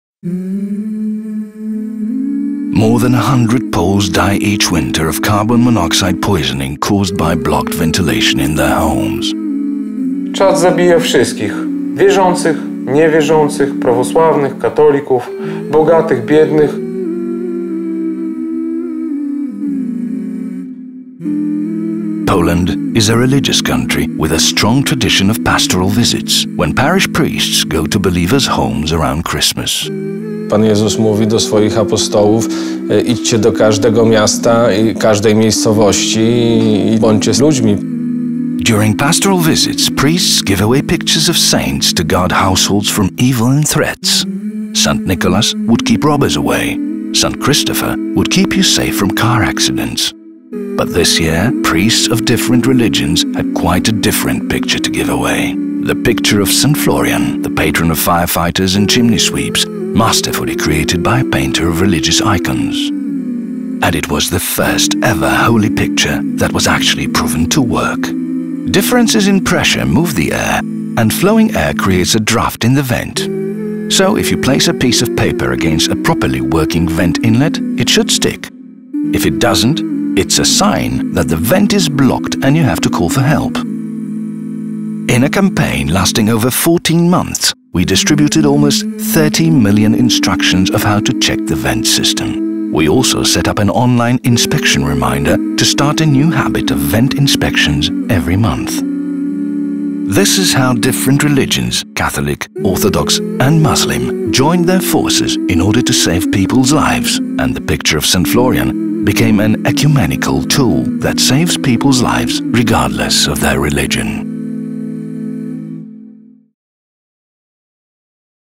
”A SIGN FROM SAINT FLORIAN” – case study film
Every year, about 100 people die in Poland by carbon monoxide poisoning.
Last year we tried to educate people with Vent Inspection Print Ad from leaflets of the largest retailers in Poland. But we could not solve the problem.
This time, to reach homes and families, we have used the Polish tradition of pastoral visits. Monoxide poisoning kills without looking at religion or social status.
This new unique campaign connects people in an ecumenical way to teach how to act proactively.